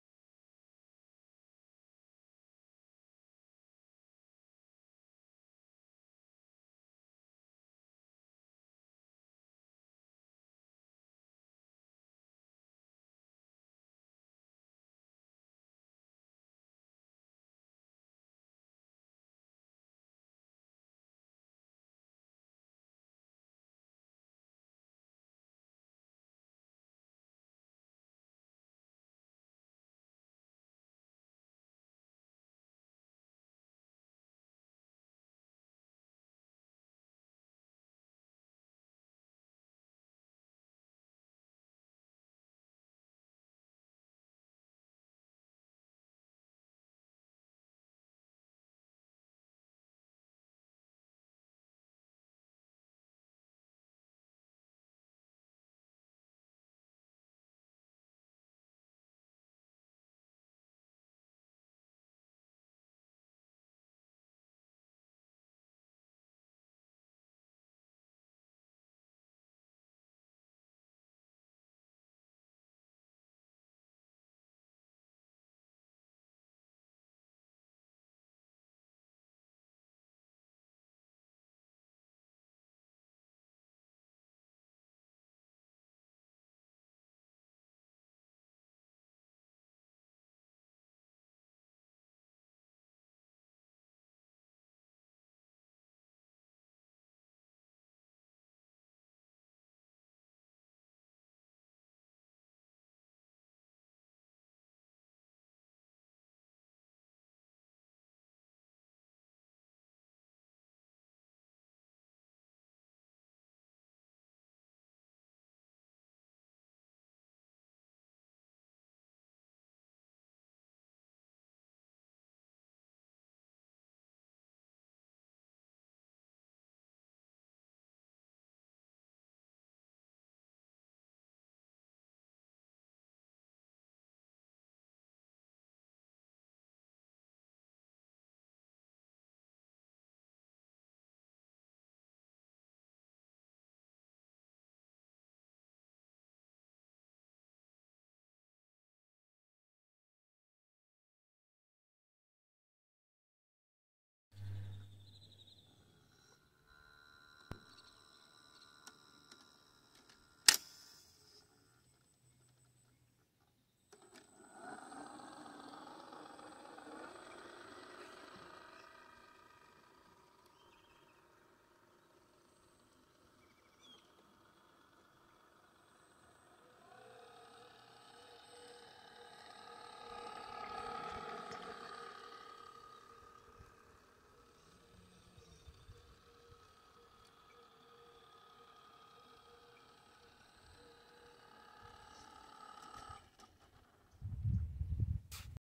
Tamiya 1/14 Grand Hauler and Flatbed Trailer finished.
Final reveal of this Tamiya Grand Hauler. A very fun build. The truck is painted with Tamiya TS-89 Pearl Blue. I lightly stained the wood pieces on the trailer.